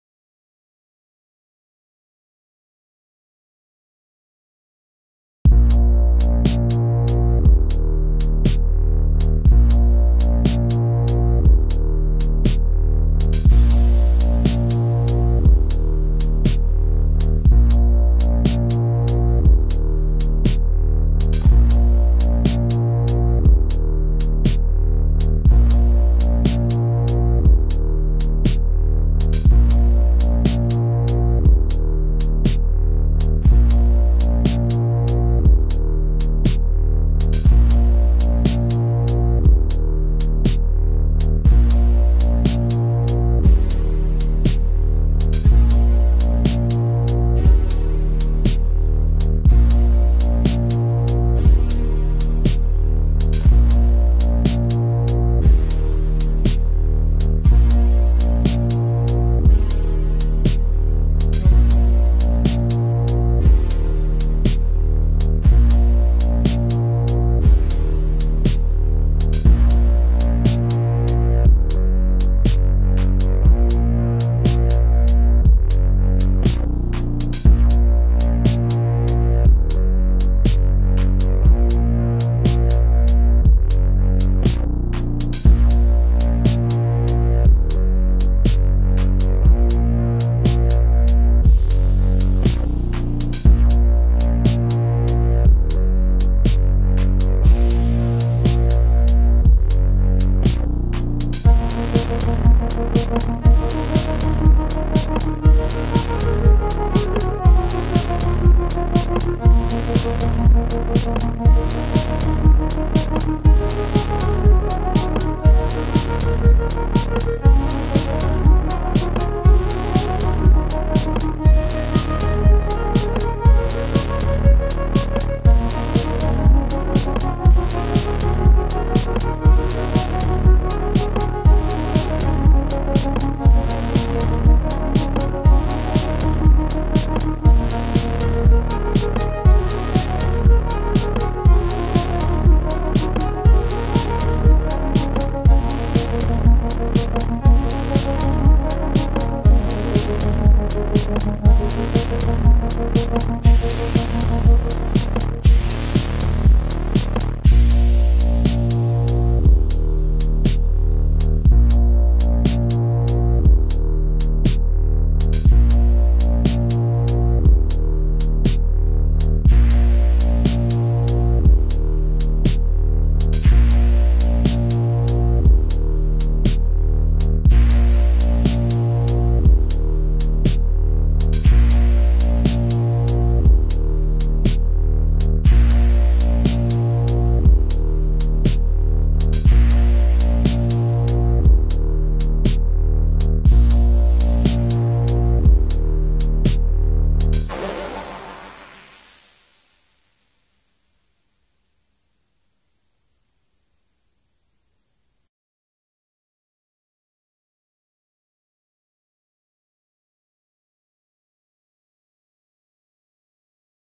Synthetic (with minibrute and OP-1)
Live improvisation with Arturia Minibrute, NI Maschine Mikro Mk2 and TE OP-1